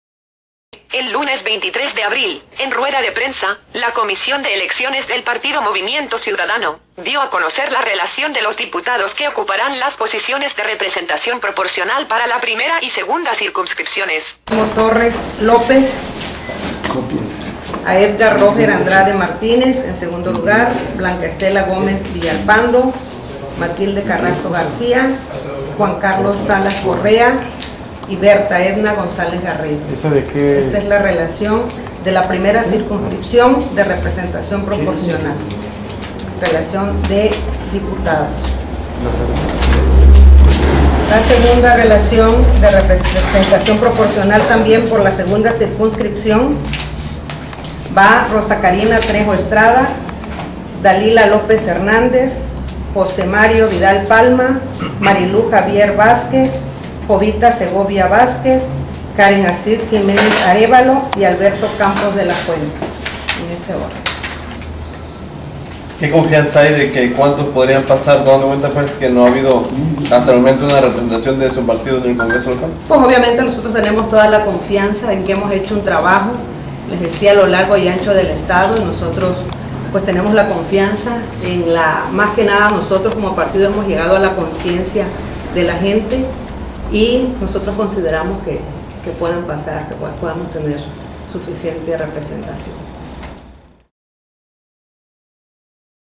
Lista candidatos a diputados de representación 1ra y 2da circunscripción PMC - Teresa Patiño.mpg
El Lunes 23 de abril, en rueda de prensa, la comisión de elecciones del Partido Movimiento Ciudadano, dio a conocer la relación de los diputados que ocuparán las posiciones de representación proporcional para la primera y segunda  circunscripciones. La lista fue presentada por los miembros de la Comisión Operativa del Partido Movimiento Ciudadano Teresa Patiño Gómez y Eduardo Salinas Pérez en el salón de usos múltiples de las instalaciones del Instituto Político.